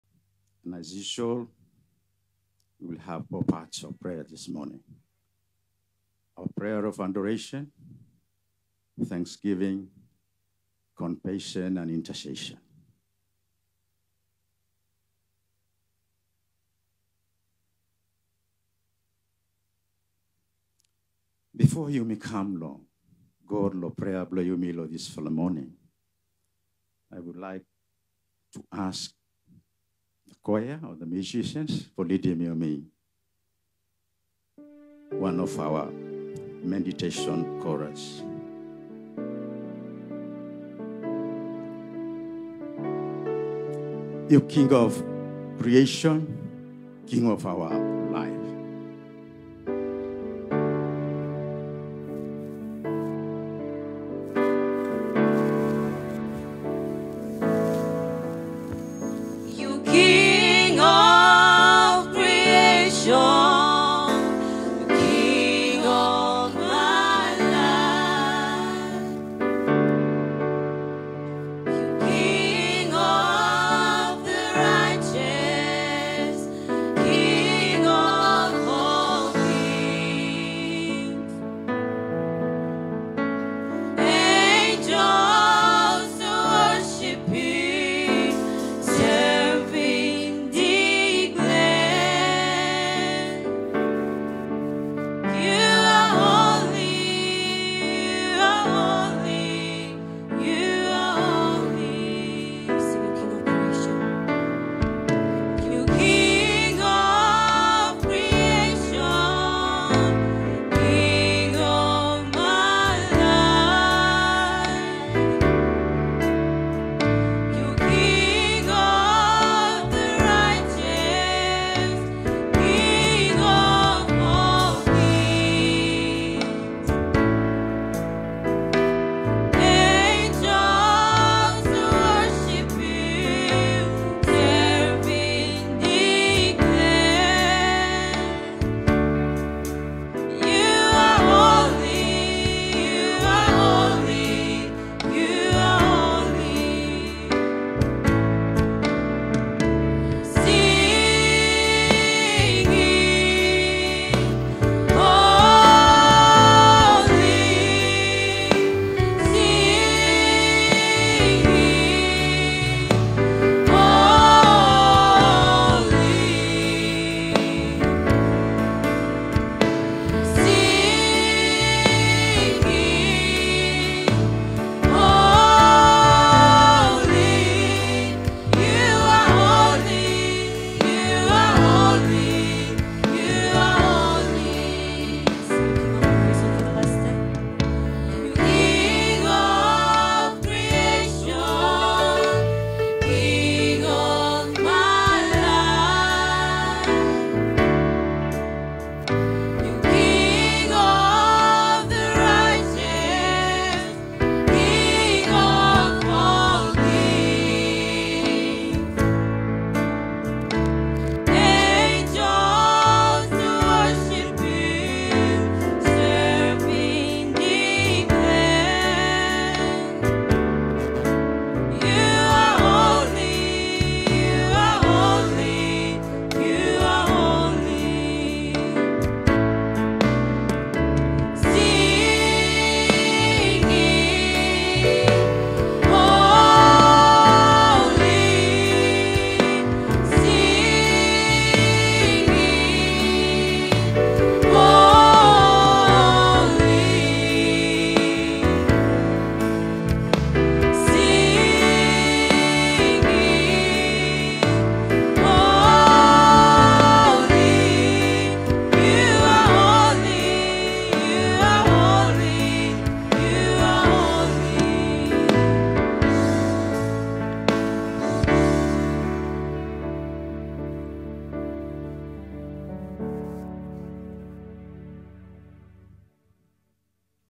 Prayer Points and Meditation Song l RC Nicholson College (22/10/23) Service, Solomon Islands
Owned by the United Church of Solomon Islands (UCSI), RC (Reginald Chapman) Nicholson College, formerly Vonunu National Secondary School, is named after the first Australian Methodist Missionary to Vella La Vella in the Western Province of Solomon Islands. Dedication and Unveiling of the new Name was presided over by the UCSI Moderator Reverend Neil Armstrong Pitakaji on Friday 29th September 2023.

Acknowledging Revealpasifik media pro for their great work in recording the Sunday Service at RC Nicholson College on Sunday 22nd October 2023.